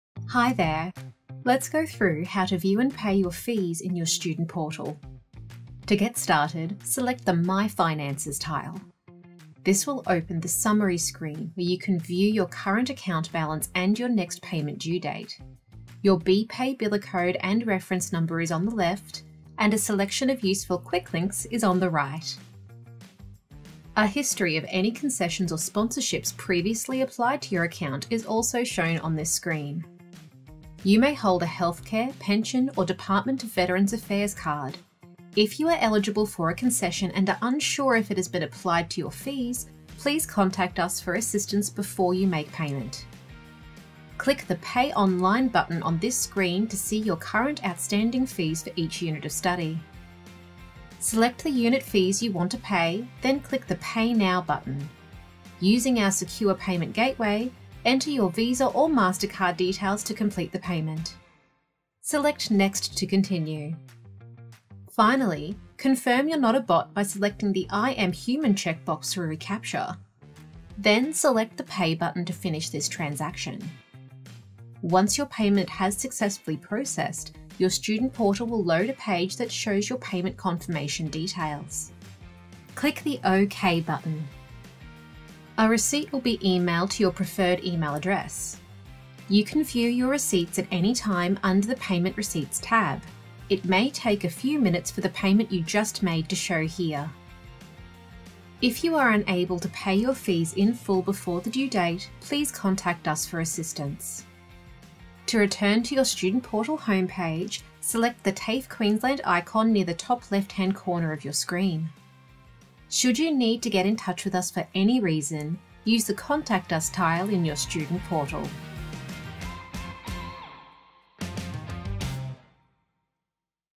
TAFE Queensland Student Portal | How to view and pay fees
The TAFE Queensland Student Portal is your one-stop shop to access the information you need about your study with TAFE Queensland. You can log in to the Student Portal here:

Watch this video to learn how to pay for unit fees in your Student Portal using a computer.

See how you can:

00:08 – Click the My Finances tile

00:11 – Take a tour of the Summary screen (current balance, due dates, BPay information, Quick Links, History of Concessions and Sponsorships

00:30 – Confirm with us that any eligible Concessions have been applied before payment

00:44 – Click the ‘Pay Online’ button

00:50 – Select the unit fees you want to pay, then the ‘Pay Now’ button

00:55 – Use our secure gateway to add your card details for payment

01:06 – Confirm your payment

01:15 – View your payment confirmation

01:25 – View your receipts in your email and the ‘Payment Receipts’ tab.

Credits and Copyright

Featured video, images and music used under license by TAFE Queensland.